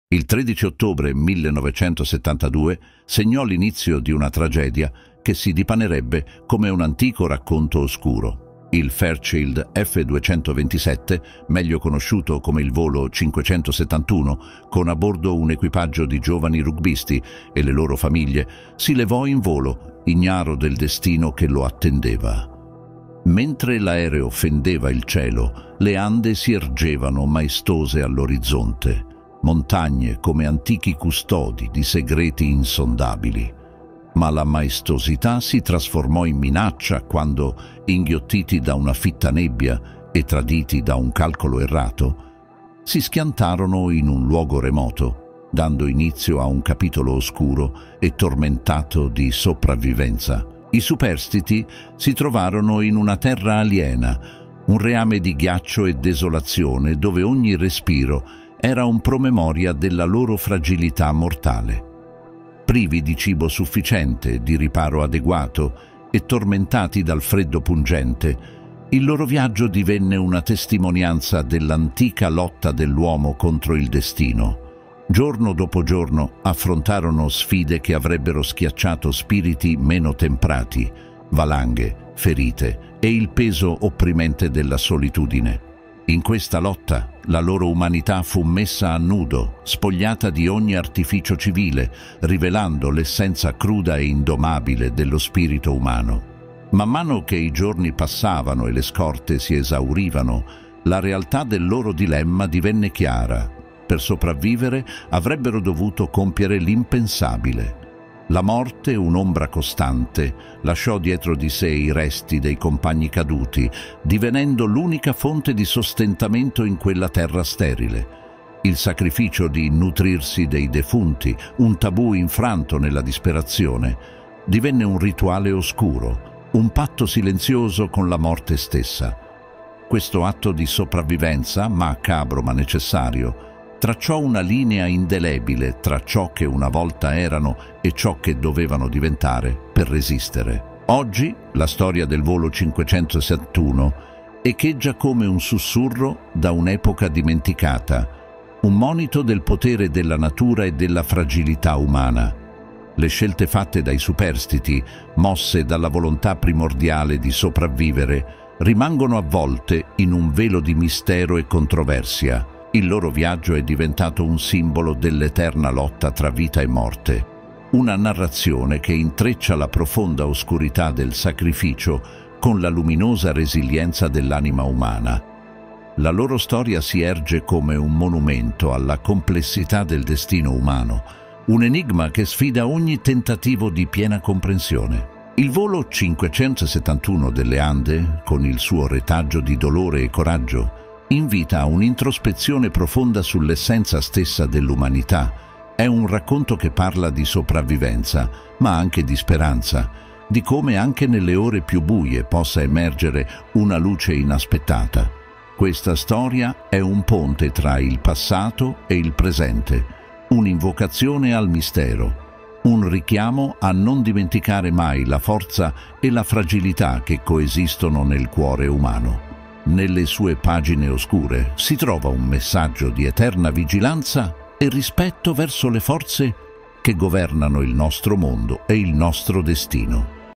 Sopravvivenza e Sacrificio
Nel cuore delle imperscrutabili Ande, si cela una storia avvolta nel mistero e nella tragedia. Il volo 571 delle Ande non è solo un racconto di disperazione e sopravvivenza, ma un inno oscuro al sacrificio umano e alla resilienza. Questa narrazione trascende il semplice racconto di sopravvivenza, trasformandosi in un viaggio attraverso la psiche umana, dove il confine tra la vita e la morte si sfuma in un'oscura danza tra etica e necessità. I superstiti, trasformati dall'antica lotta contro il destino, emergono non solo come figure di coraggio, ma come simboli di un enigma più profondo che risiede nell'anima umana. La storia del volo 571 è un invito a sondare le profondità del mistero, un'eco persistente che risuona con domande insondabili sulla natura della vita, del sacrificio e della forza interiore.